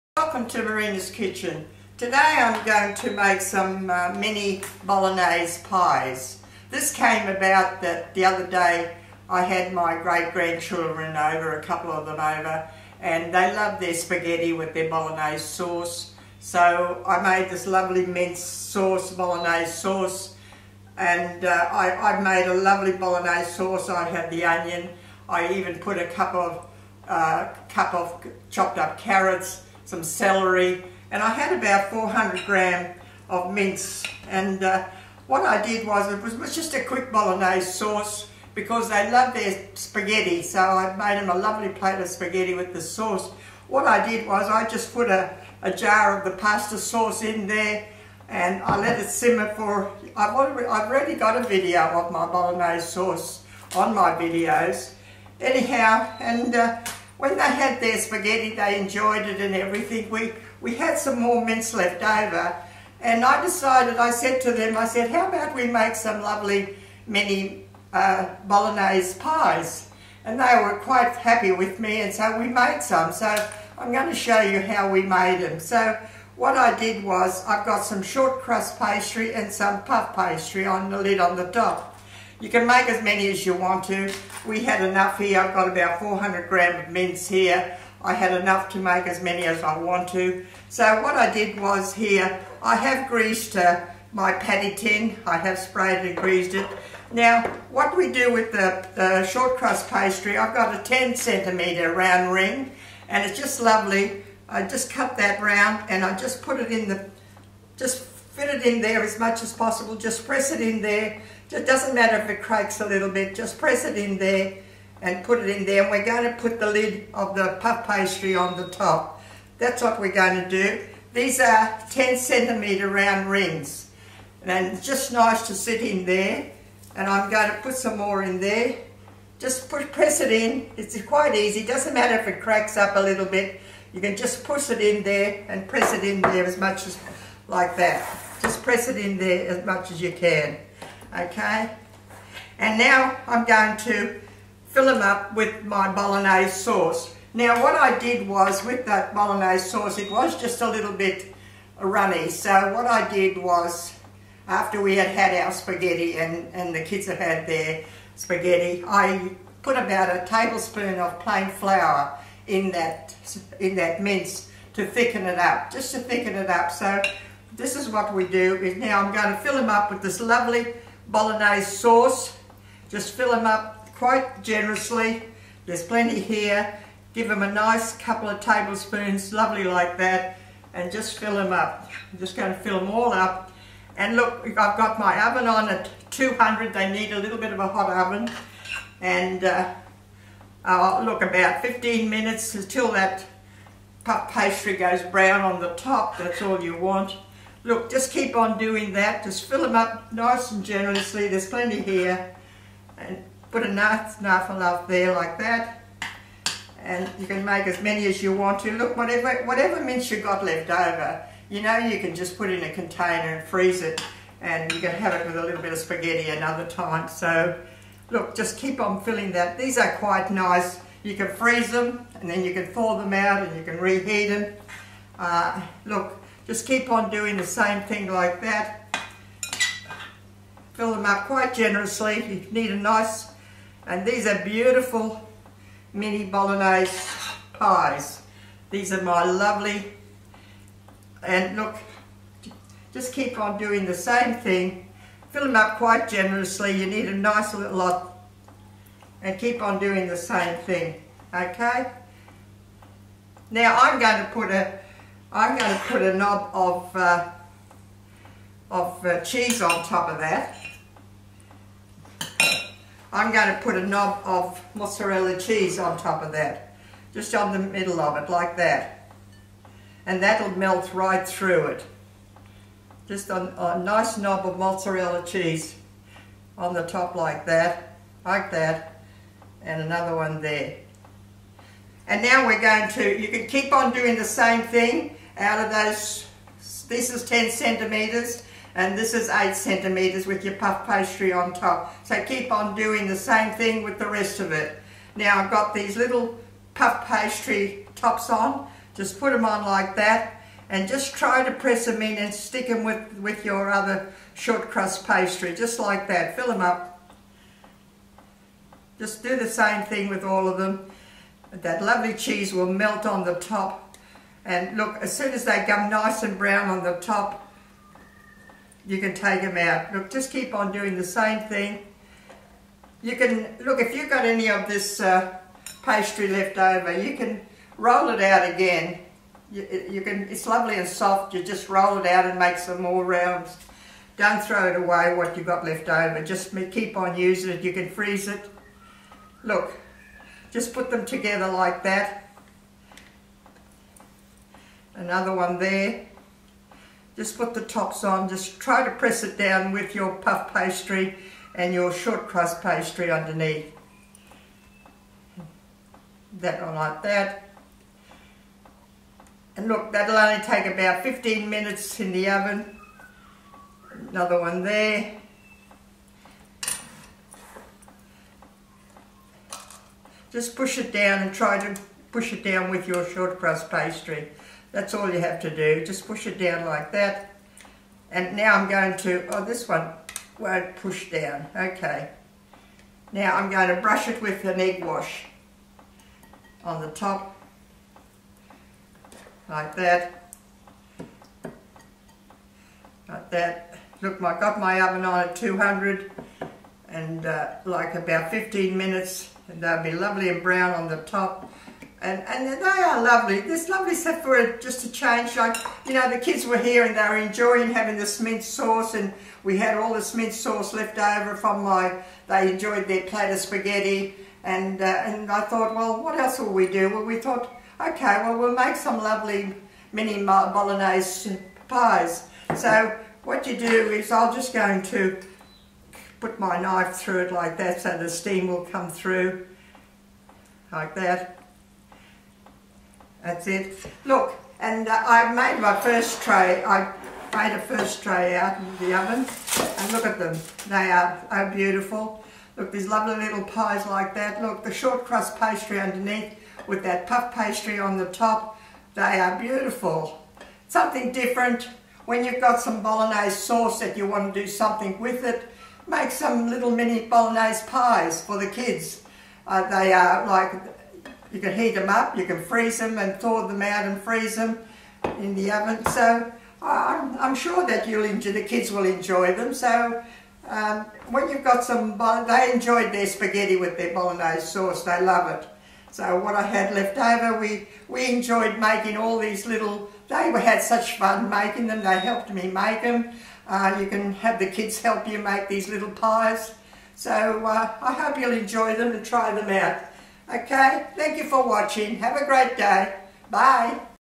MARINA'S KITCHEN ----MINI BOLOGNESE PIES-
Bolognese sauce about 500 g --18  round of  10 cm short crust pastry --18 round 8 cm puff pastry ---mozzarella cheese -   200 deg for 15 to 20 mins